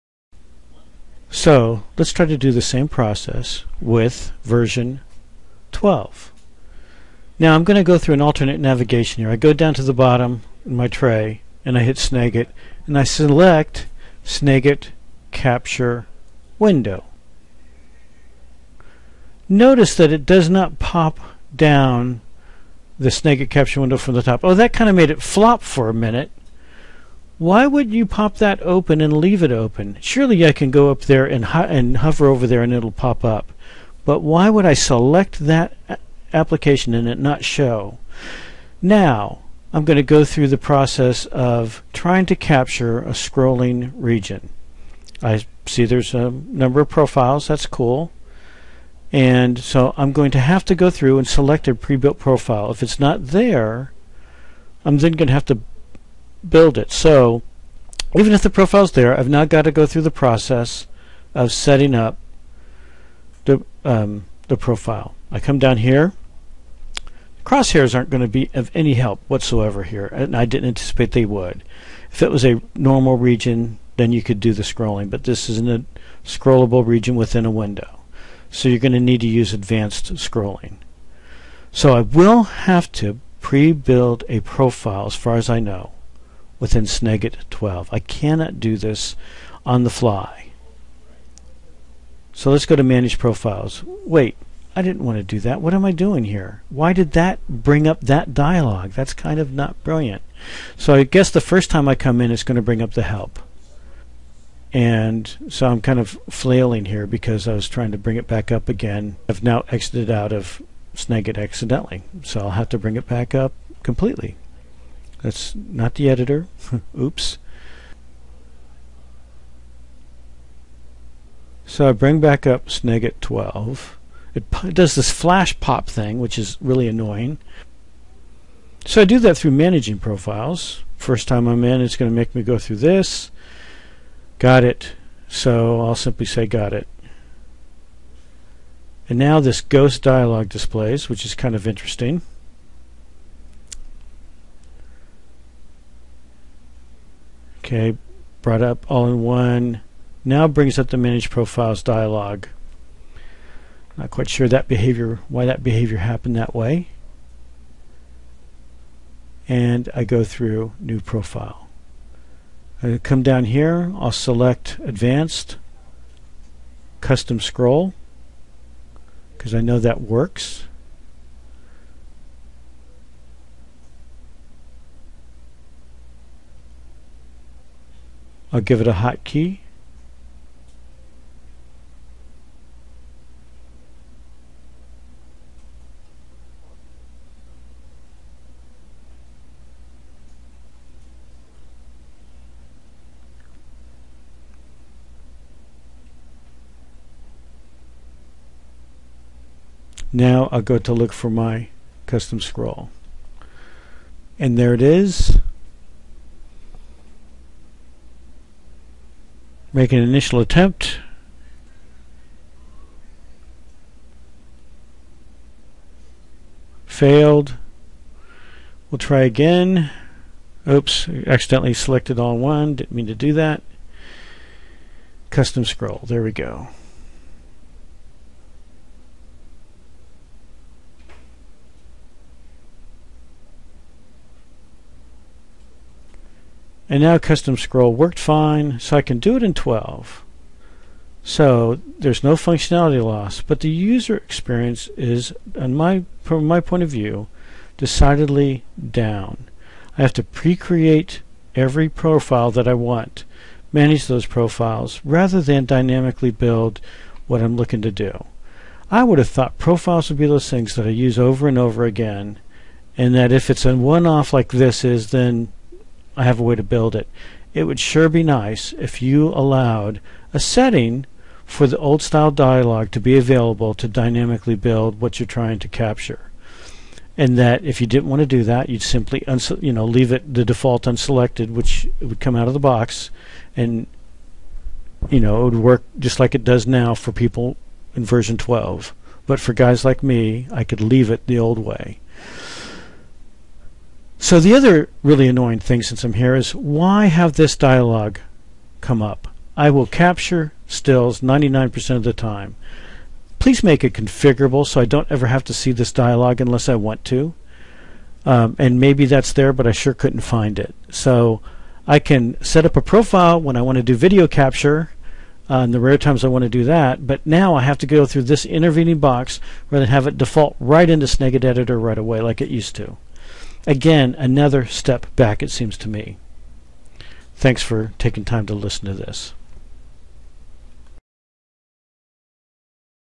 Snagit 12 Advanced Scrolling - Custom
Walk through capturing a scrolling part of a window in Snagit 12 version